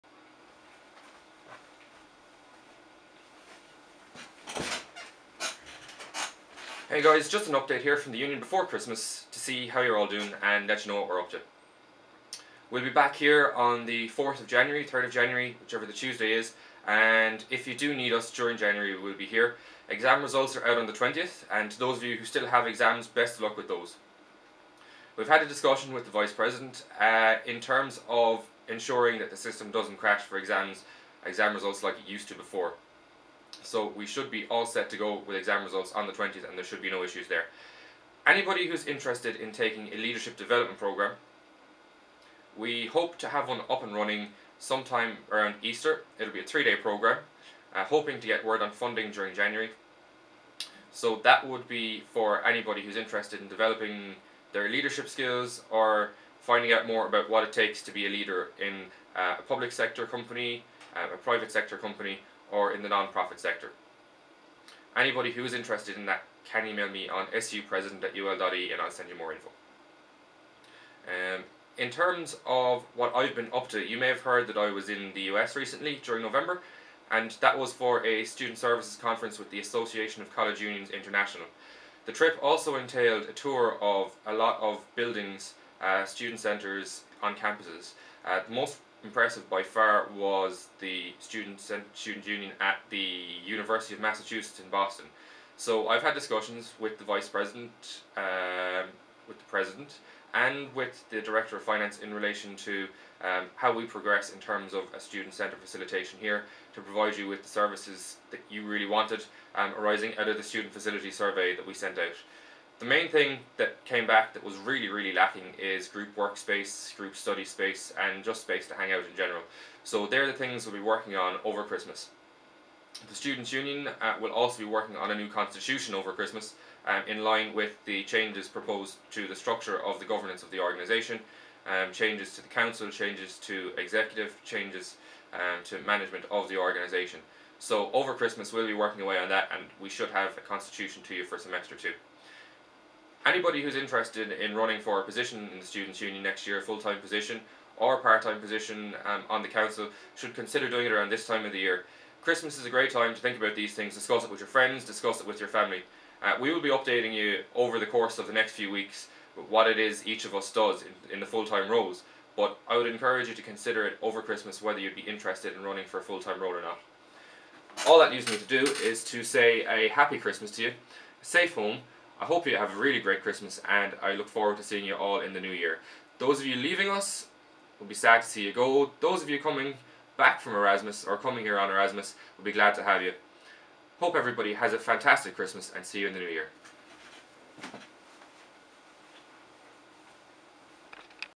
Happy Christmas 2011 from ULSU
Wishing you all a happy Christmas, best of luck in exams a fond farewell and a welcome home.